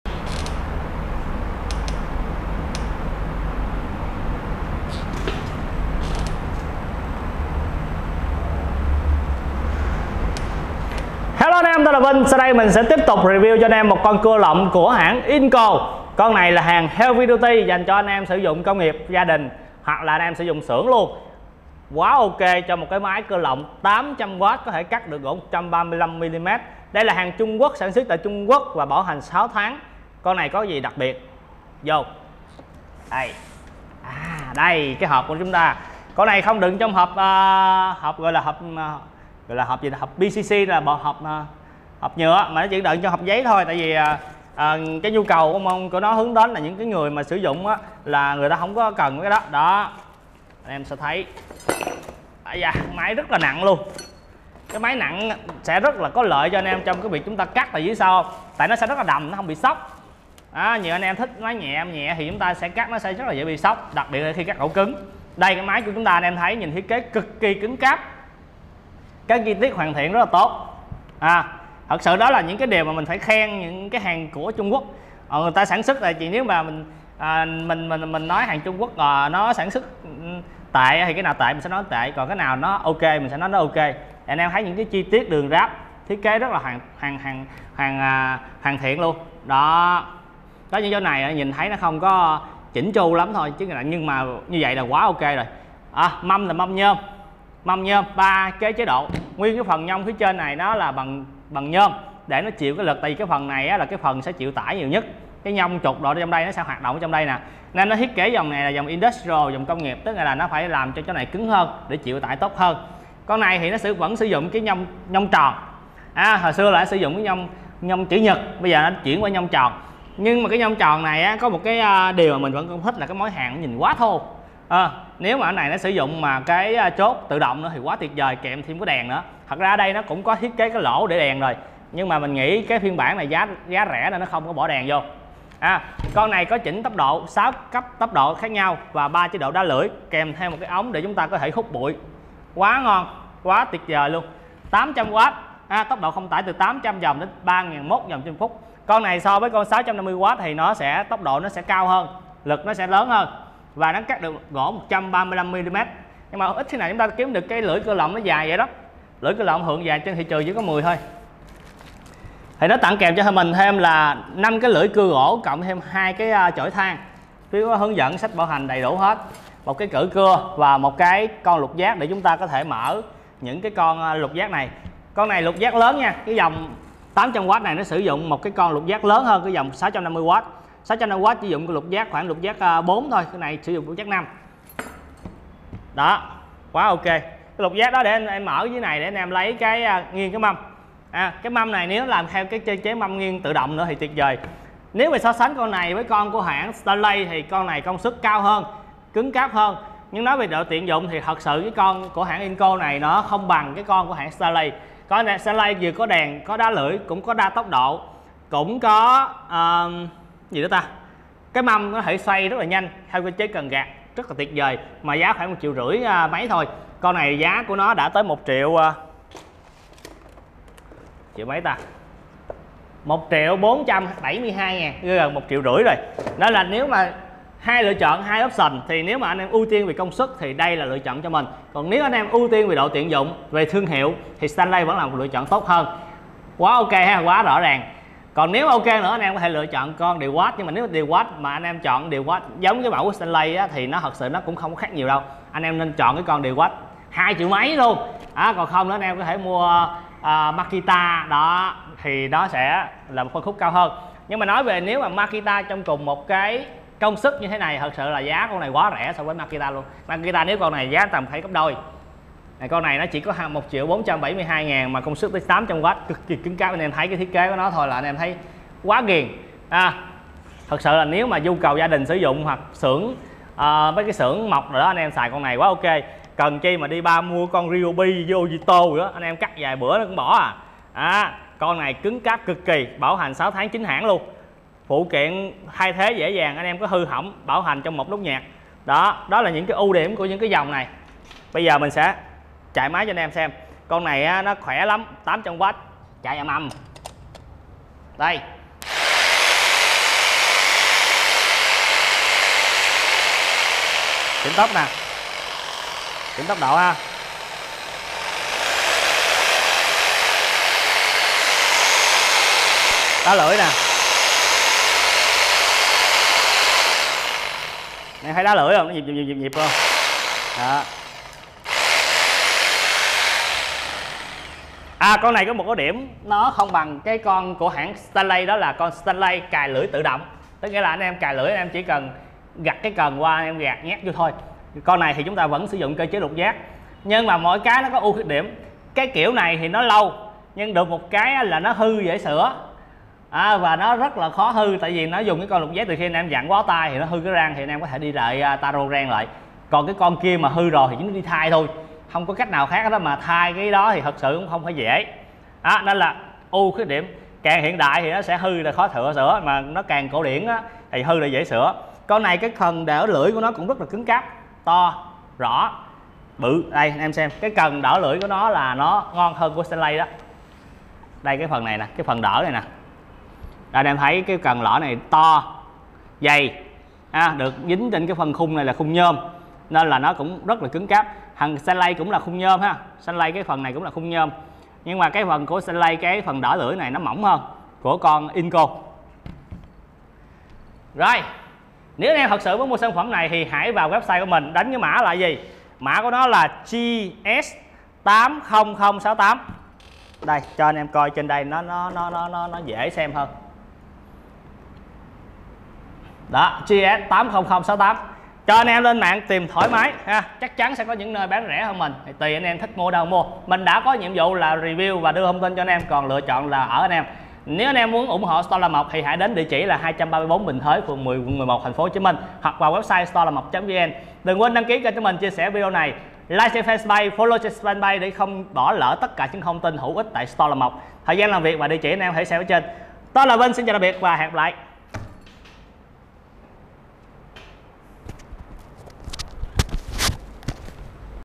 REVIEW MÁY CƯA LỌNG 800W INGCO JS80068
INGCO JS80068 | MÁY CƯA LỌNG 800W
Điện thế: 220-240V~50-60Hz
Công suất: 800 W
Tốc độ không tải: 800-3100 rpm
Khả năng cắt gỗ: 135mm thép: 10mm,
Kèm theo 5 lưỡi cưa và 1 bộ carbon

chỉ có ở
Mua hàng tại: 234 Bình Thới, P10, Q11, HCM
Giờ mở cửa: từ 9h-22h từ thứ 2 đến thứ 7   RIÊNG CN từ 9h đến 18h
Bằng cách đăng ký miễn phí và nhấn chuông thông báo.